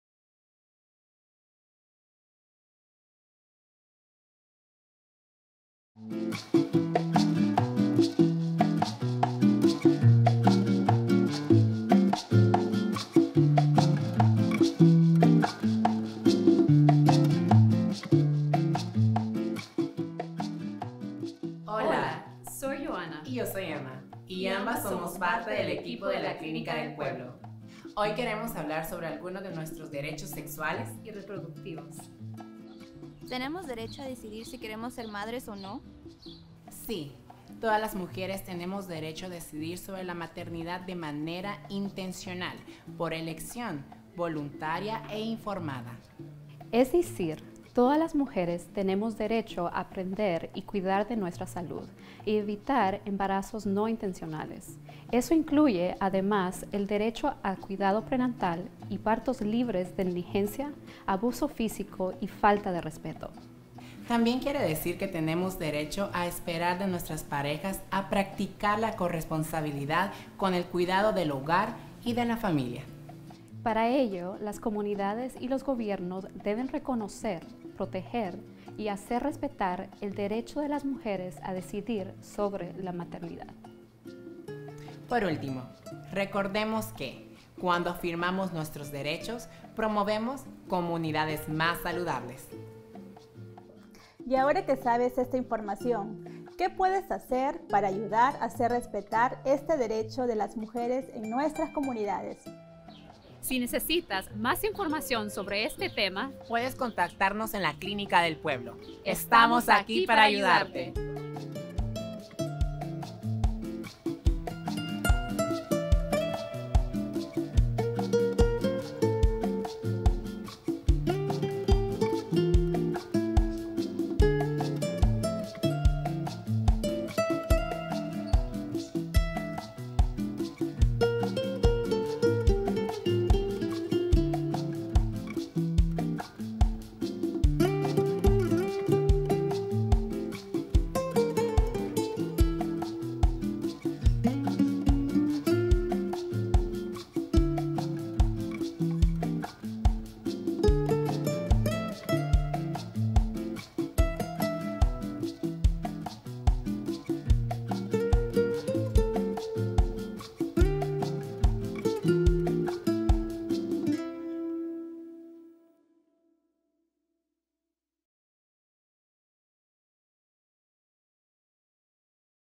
Maternidad Intencional
Todas las mujeres tenemos derecho a decidir sobre la maternidad de manera intencional, por elección, voluntaria e informada.
Recordemos que, cuando afirmamos nuestros derechos, promovemos comunidades más saludables.

Our public health campaign reminds us that all women have the right to decide about motherhood intentionally, by choice, voluntarily and in an informed manner.
When we affirm our rights, we promote healthier communities!